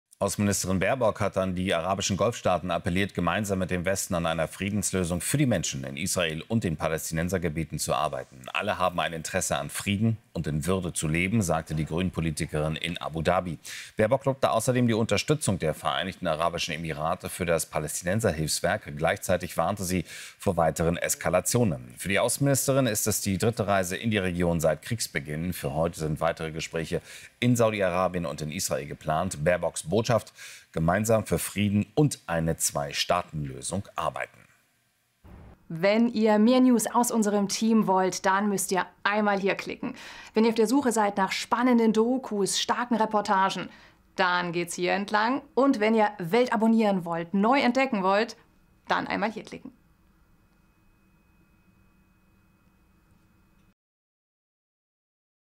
ANNALENA BAERBOCK APPELIERT IN ABU DHABI: Gemeinsam für Frieden in Israel und Palästinensergebieten
BAERBOCK APPELIERT IN ABU DHABI: Gemeinsam für Frieden in Israel und Palästinensergebieten

Annalena Baerbock hat an die arabischen Golfstaaten appelliert, gemeinsam mit dem Westen an einer Friedenslösung für die Menschen in Israel und den Palästinensergebieten zu arbeiten.
»Alle Menschen haben ein Interesse an Frieden und daran, in Würde zu leben. Alle Menschen haben ein Recht, in Frieden und Würden zu leben«, sagte die Außenministerin in Abu Dhabi. Baerbock hat außerdem die Unterstützung der Vereinten Arabischen Emirate für das Palästinenserhilfswerk gelobt, gleichzeitig aber auch vor weiteren Eskalationen gewarnt. Für die Außenministerin ist es die dritte Reise in die Region seit Kriegsbeginn. Für Samstag sind weitere Gespräche in Saudi-Arabien und Israel geplant. Ihre Botschaft: Gemeinsam für Frieden und eine Zwei-Staaten-Lösung arbeiten.